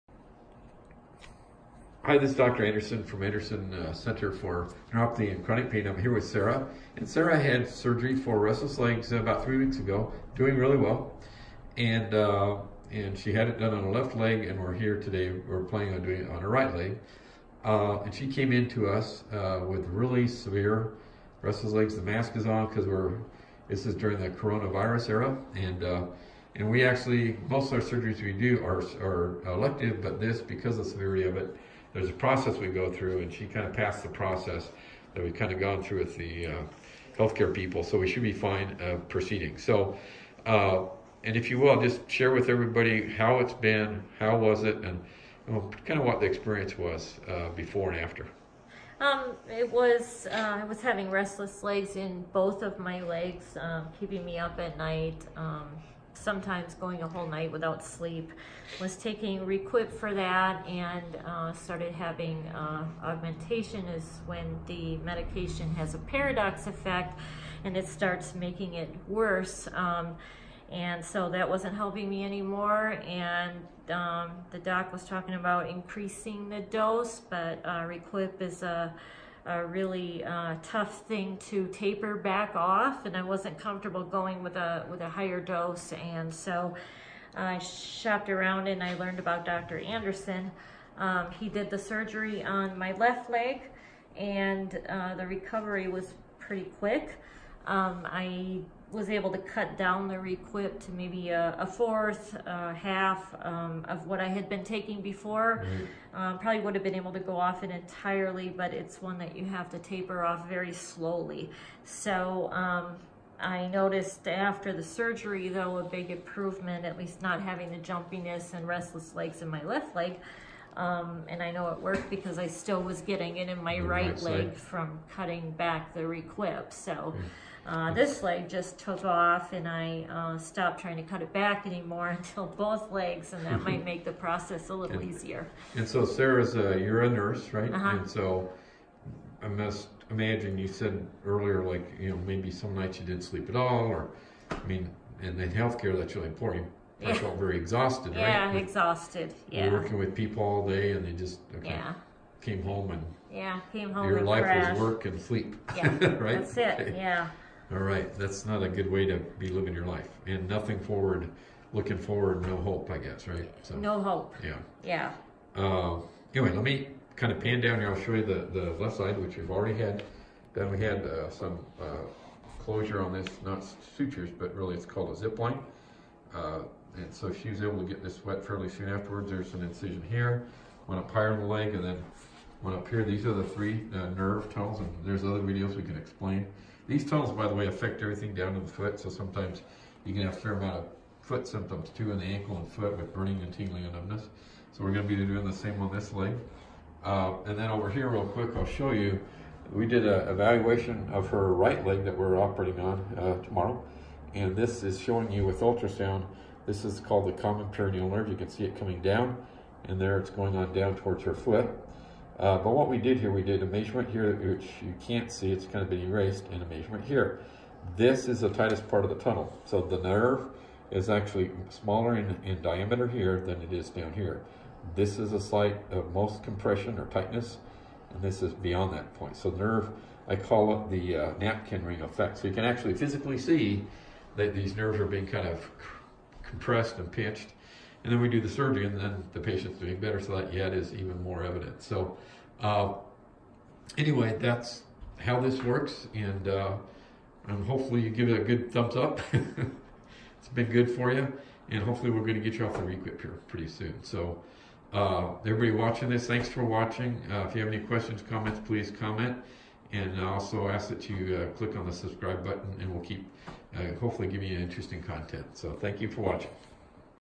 Restless Legs reversed, no more Requip ! You'll see ultrasound evidence of nerve compression.
You meet Sarah and learn about her quick reversal of RLS . I'll also show how the appearance of a nerve  on ultrasound has its shape changed by severe compression on it.